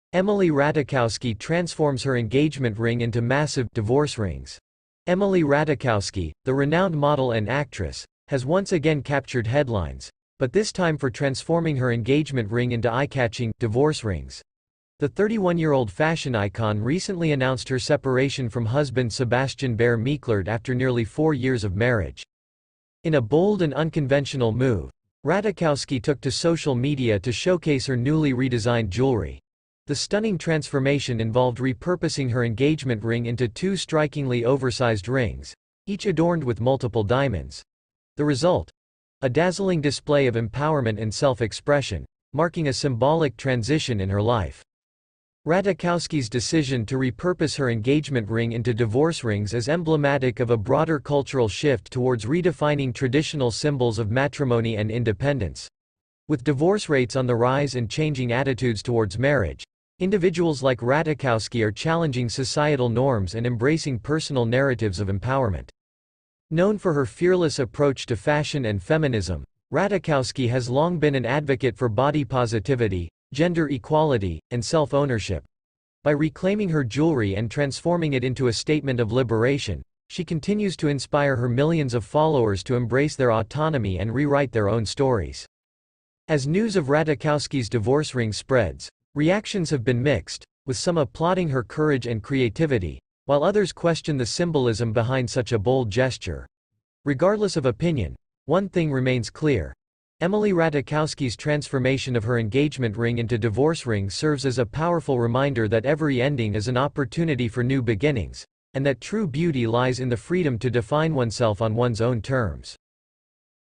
Emily Ratajkowski Transforms Her Engagement Ring Into Massive "Divorce Rings"
Emily Ratajkowski, the renowned model and actress, has once again captured headlines, but this time for transforming her engagement ring into eye-catching "divorce rings." The 31-year-old fashion icon recently announced her separation from husband Sebastian Bear-McClard after nearly four years of marriage.

In a bold and unconventional move, Ratajkowski took to social media to showcase her newly redesigned jewelry. The stunning transformation involved repurposing her engagement ring into two strikingly oversized rings, each adorned with multiple diamonds. The result? A dazzling display of empowerment and self-expression, marking a symbolic transition in her life.